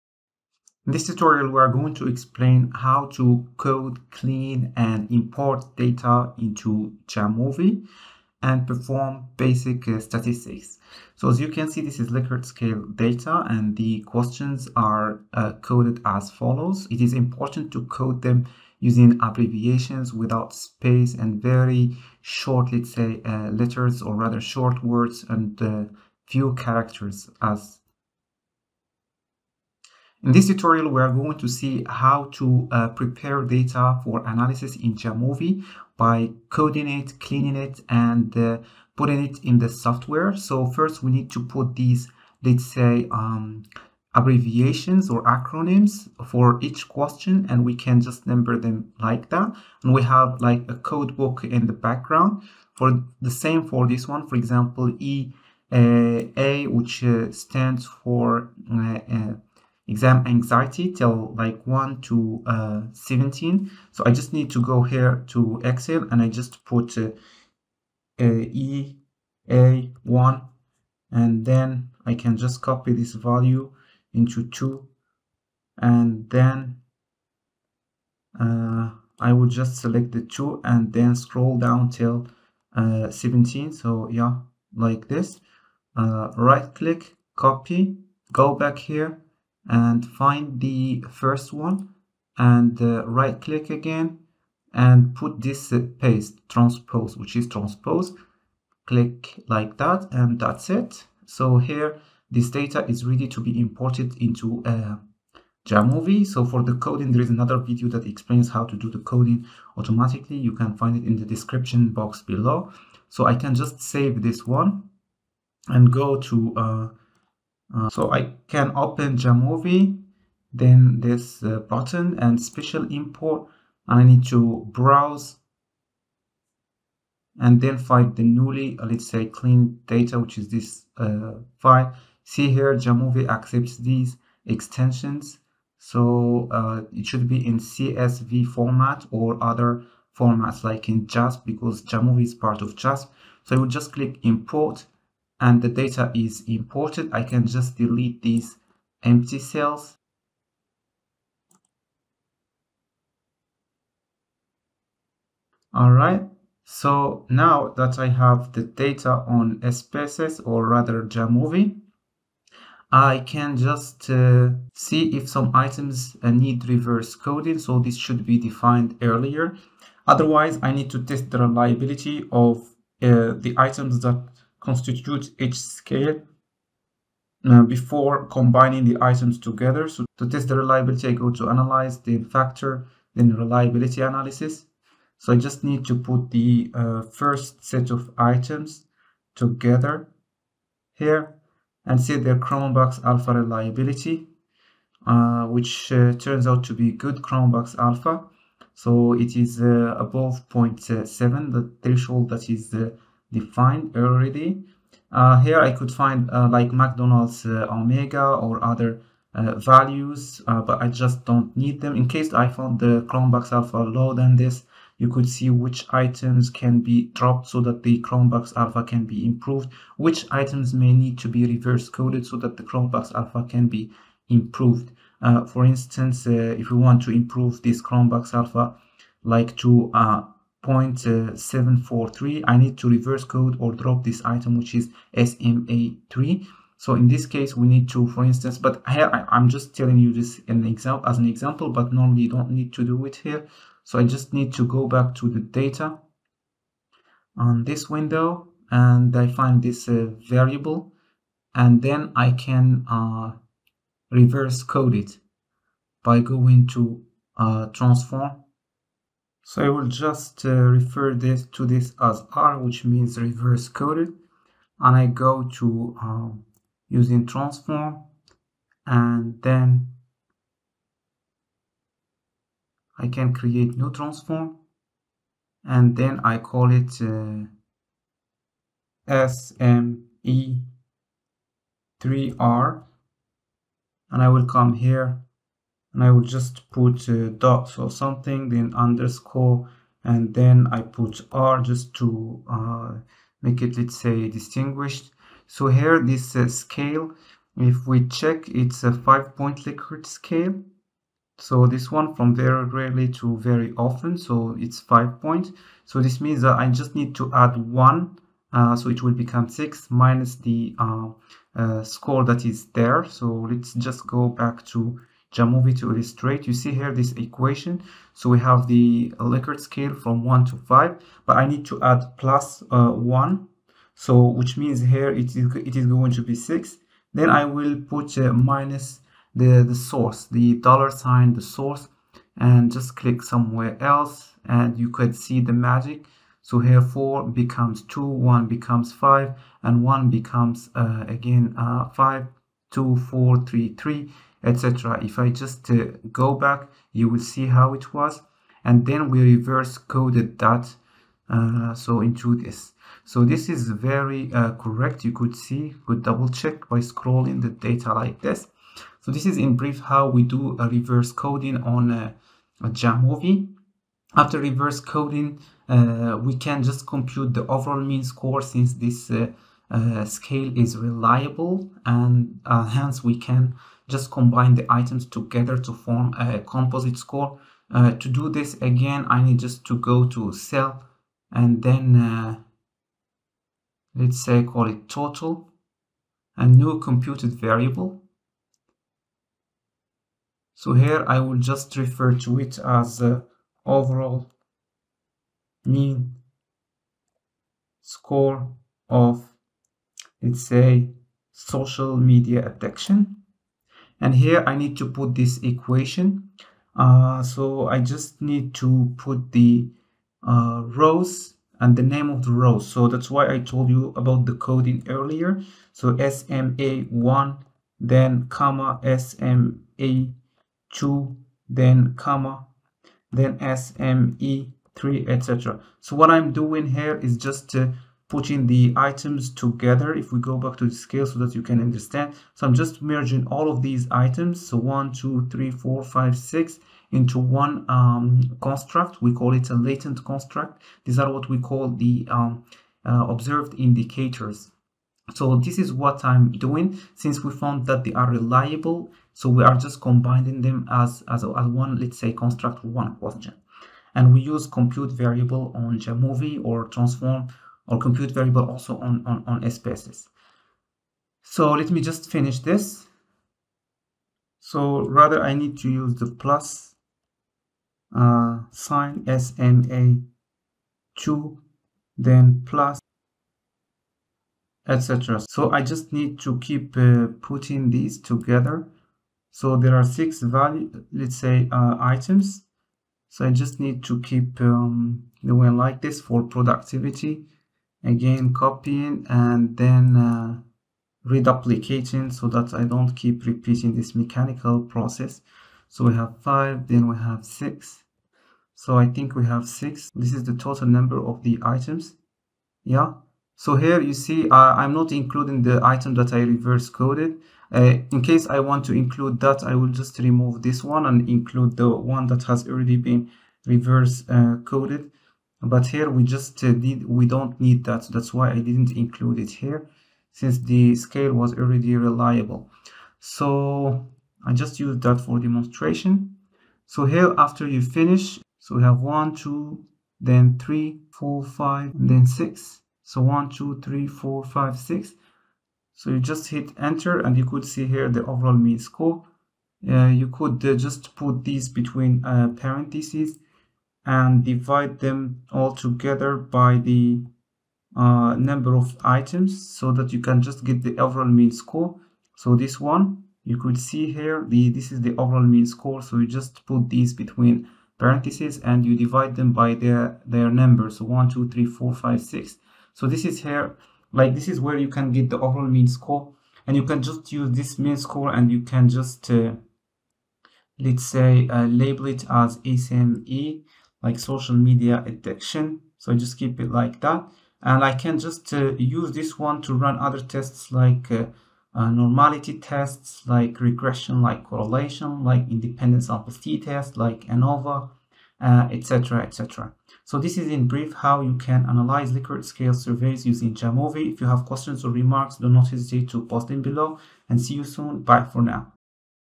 The best ways to analyse Likert scale survey on Jamovi
The Likert scale data can be coded from 1 to 5 depending on the type of the scale used. The questions or items will need to be abbreviated in short names with no spaces before being imported into Jamovi. The Cronbach alpha reliability will need to be tested. If some items show low Cronbach alpha reliability below .70, the need to be reverse coded by transform 5+1 - Dollar sign source. After that, the overall mean score will need to be computed by summing items together and dividing them by their number. Then, other hypothesis testing procedures can be applied like normality, t-test, correlation, regression and ANOVA.